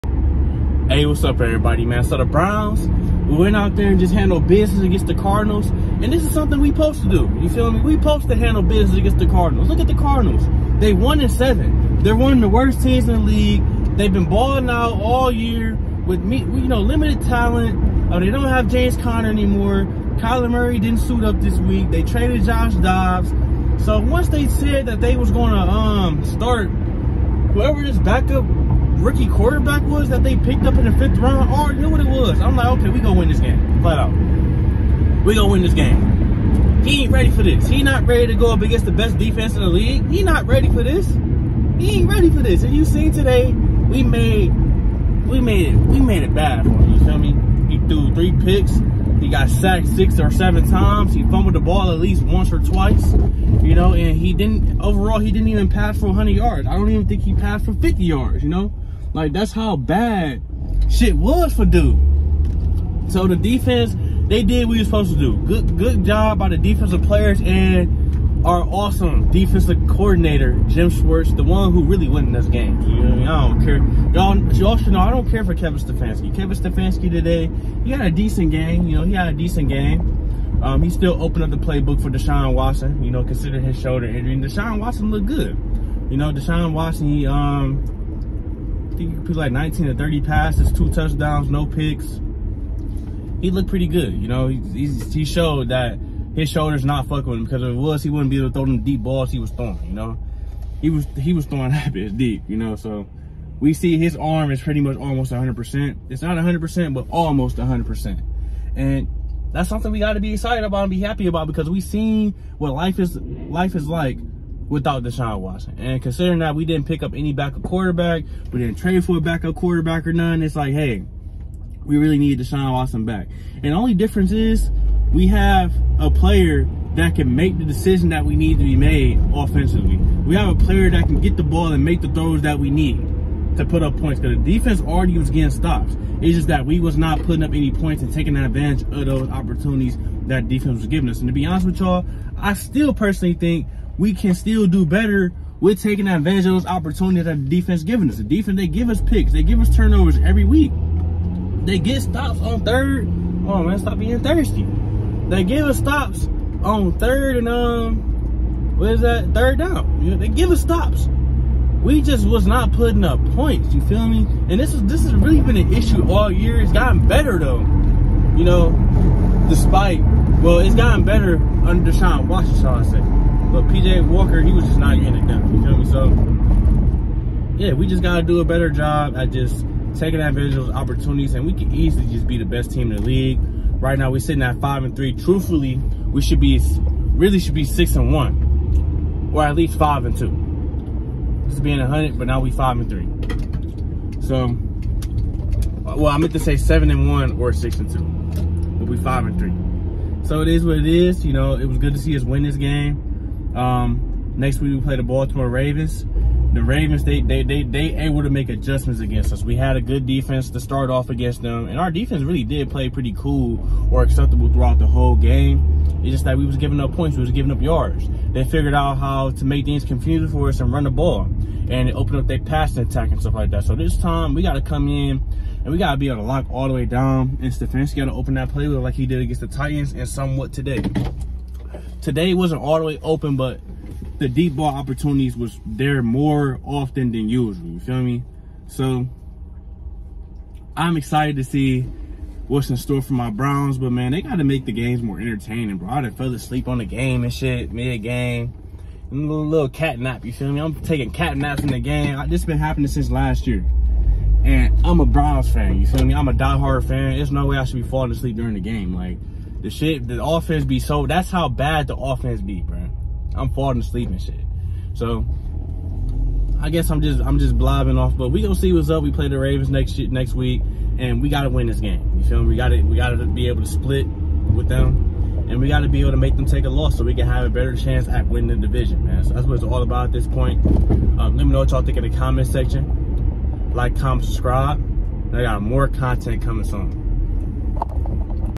Cleveland Browns Vs Arizona Cardinals | week 9 recap
The cleveland browns were able to handle business against the Arizona Cardinals at home to improve to 5-3. The cardinals decided to start their rookie qb clayton tune against the best defense in the league, which made things easier for the browns. The browns were able to make it a shut out victory. Deshaun watson looked great in his return from his shoulder injury as well. Here’s a recap